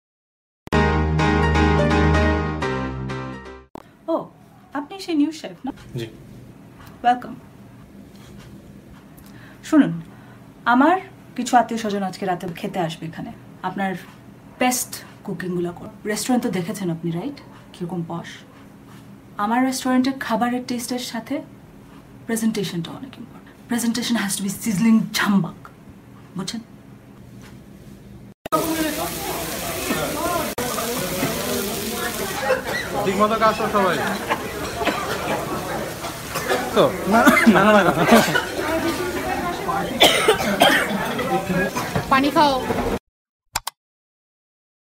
New Chef's Crazy Beef sizzling
New Chef joins Rezwana's restaurant making the ultimate Beef Sizzling to impress!
bangla বাংলাদেশী চাইনিজ বিফ সিজলিং - Bangladeshi Chinese Beef Sizzling